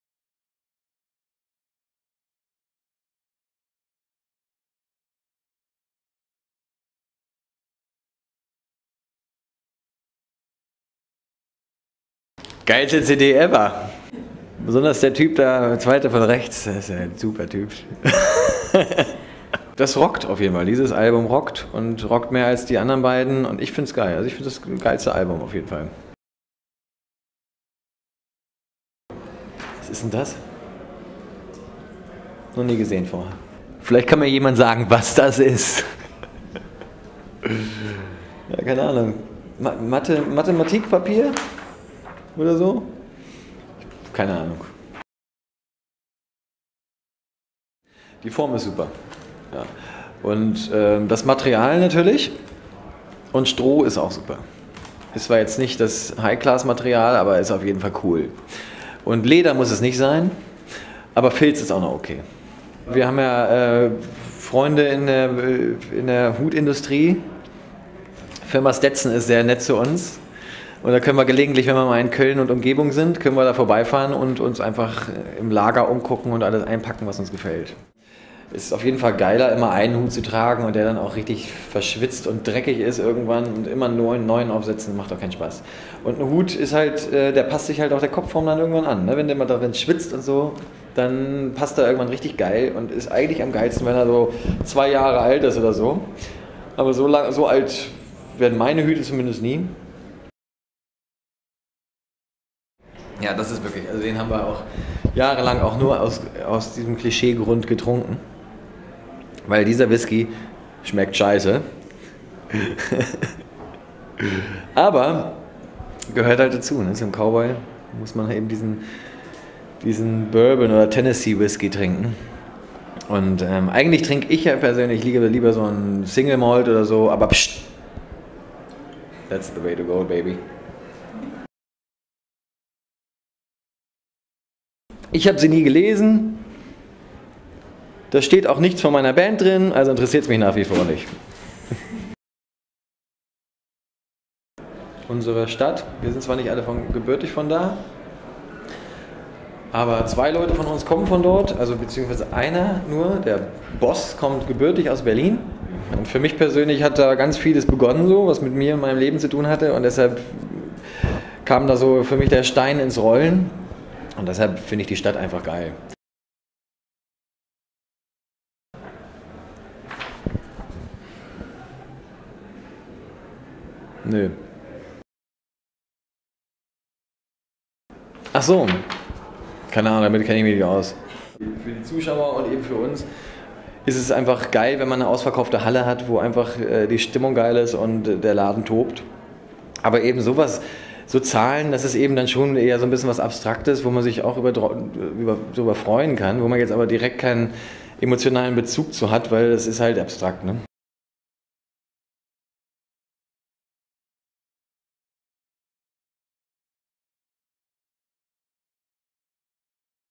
sieben sachen für frank doe von the boss hoss
ein video-interview der anderen art: anstatt potentiell langweilige fragen zu stellen, gaben wir boss hoss drummer frank doe sieben sachen in die hand. zum drüber reden.

frank doe über cowboyhüte, die bravo und berlin: enjoy!

[aufgenommen für fudder.de, 30. januar 2008, e-werk, freiburg]